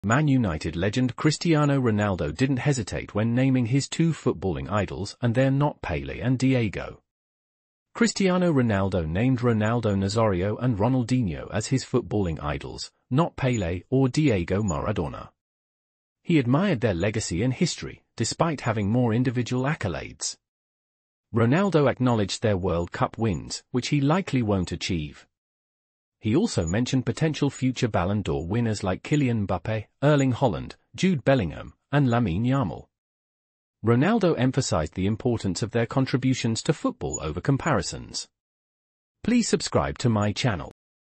Man Utd legend Cristiano Ronaldo didn't hesitate when naming his two footballing idols and they're n
Cristiano Ronaldo named Ronaldo Nazario and Ronaldinho as his footballing idols, not Pele or Diego Maradona. He admired their legacy and history, despite having more individual accolades. Ronaldo acknowledged their World Cup wins, which he likely won't achieve. He also mentioned potential future Ballon d'Or winners like Kylian Mbappe, Erling Haaland, Jude Bellingham, and Lamine Yamal. Ronaldo emphasized the importance of their contributions to football over comparisons.